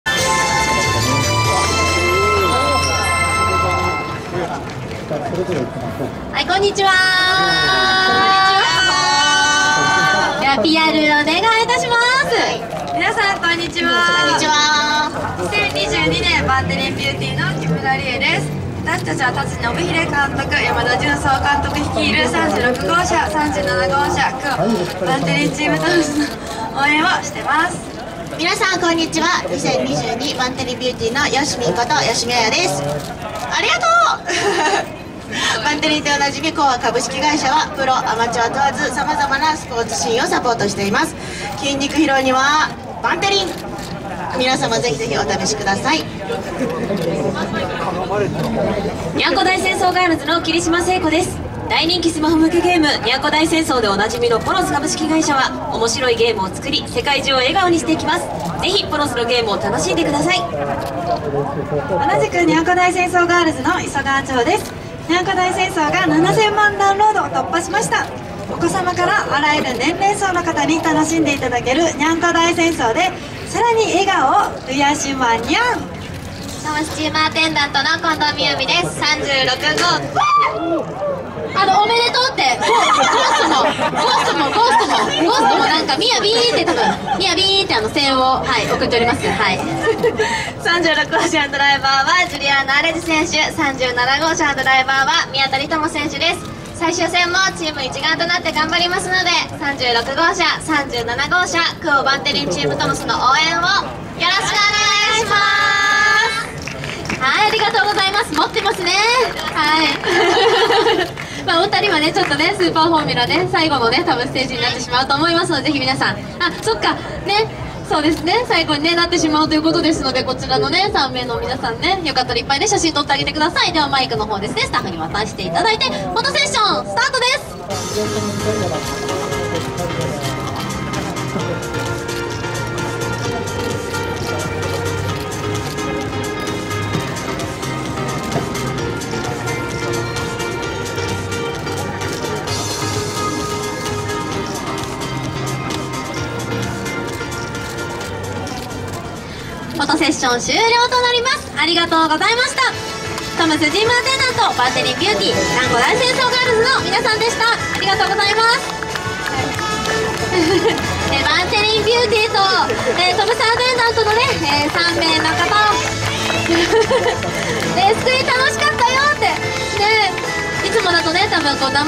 2022 SUPER FORMULA Rd.10 SUZUKA RQ03 #36 #37 近藤みやび 吉美あや 木村理恵 霧島聖子 五十川ちほ TOM'S レースクイーンステージ 4K
OM’S Team Attendant